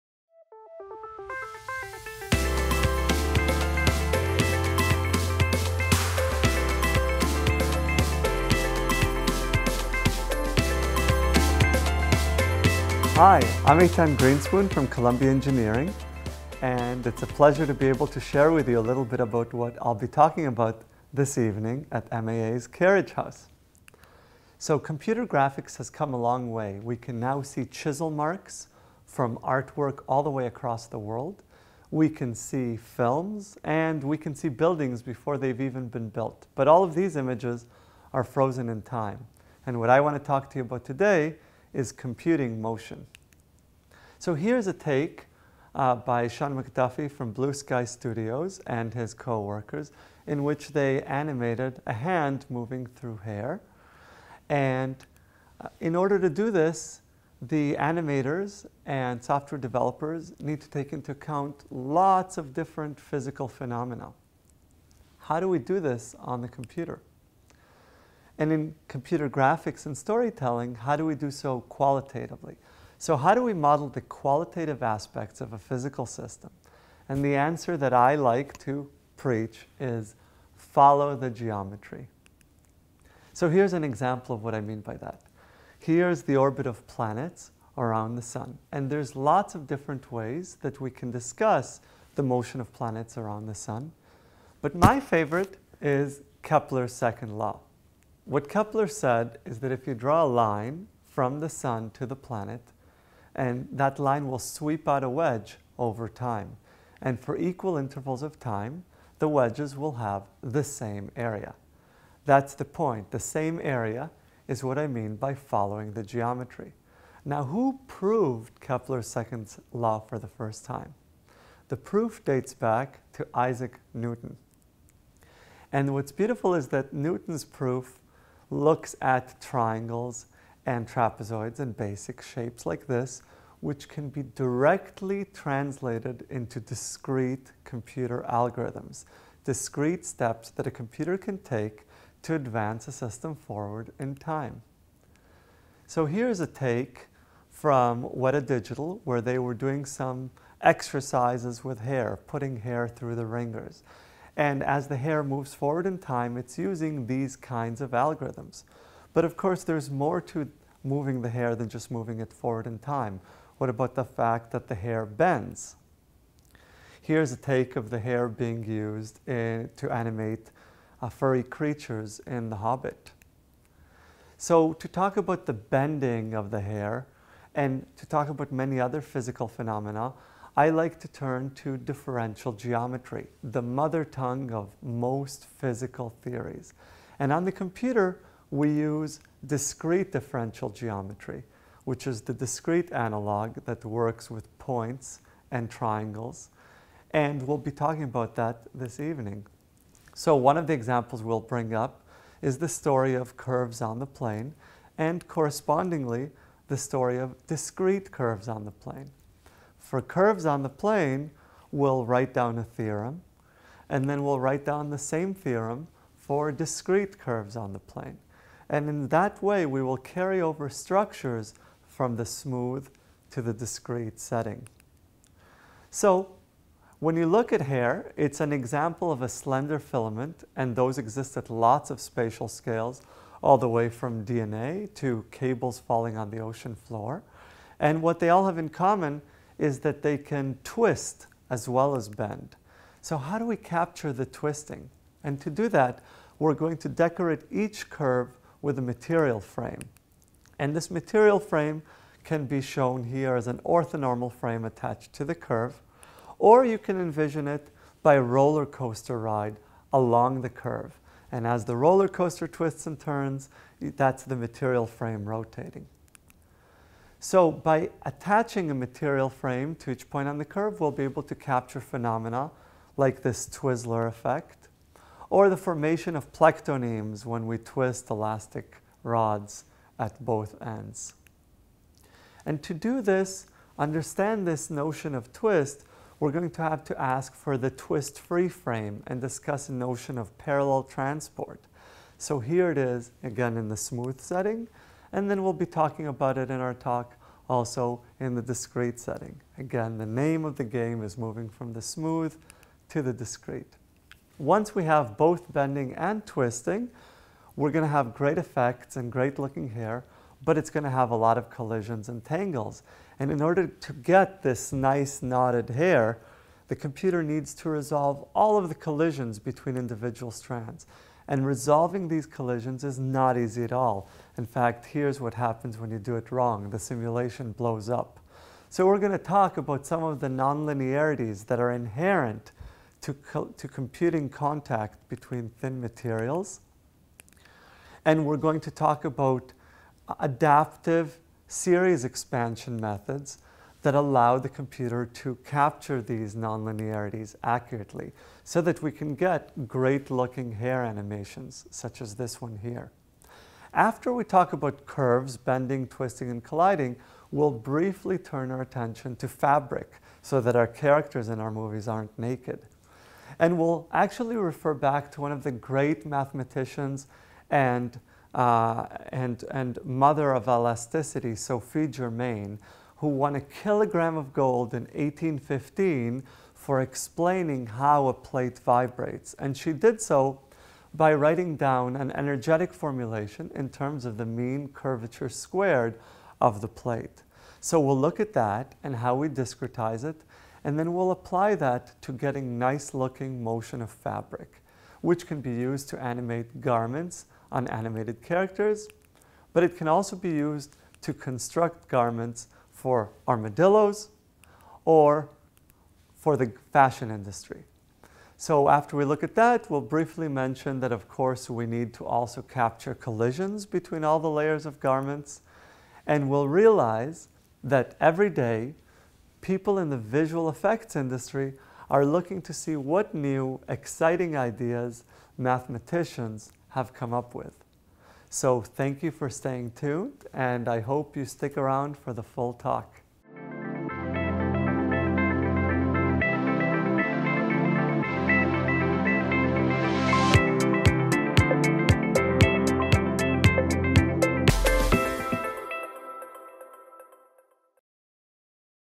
"Movie Magic: The Mathematics behind Hollywood's Visual Effects", Eitan Grinspun
Blockbuster films have amazing visual effects. Virtual stunt doubles, animated characters, and imaginary creatures are built from mathematical models of hair, fur, skin, and clothing. Explosions, floods, and disasters that would be dangerous if not impossible to film in real life are instead simulated on computers using mathematical models of fracture, fire, granular media, and liquids. This is the world of applied mathematics with an artistic flair. In this video, Eitan Grinspun will expose various aspects of movie magic, and the exciting mathematical questions that arise.